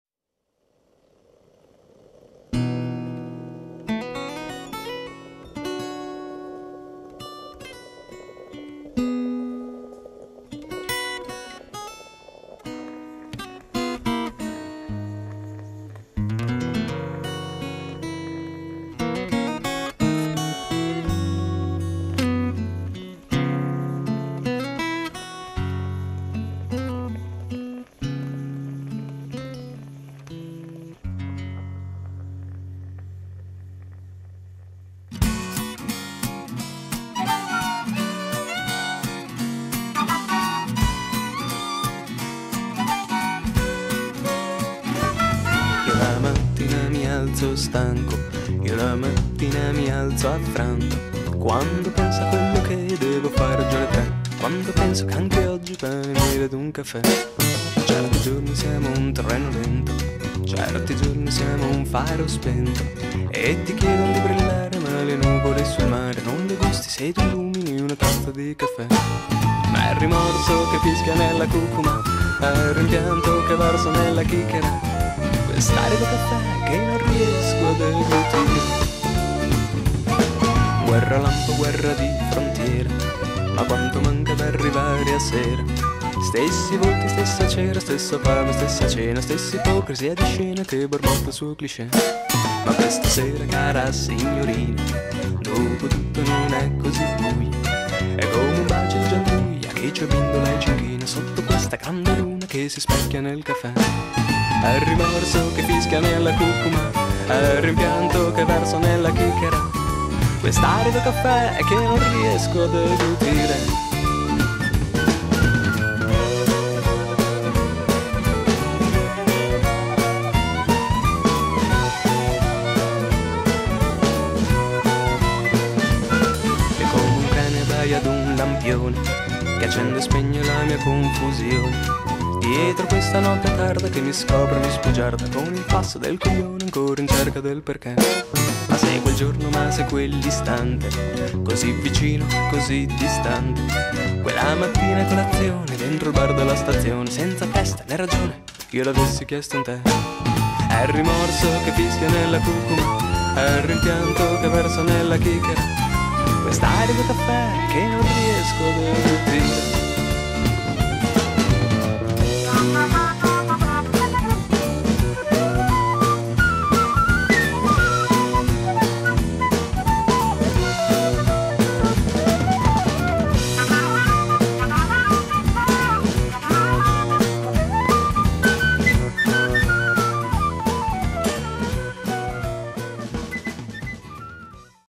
Caffè dei treni persi
Caffè dei treni persi incidono all'Esagono di Rubiera - visit my myspaces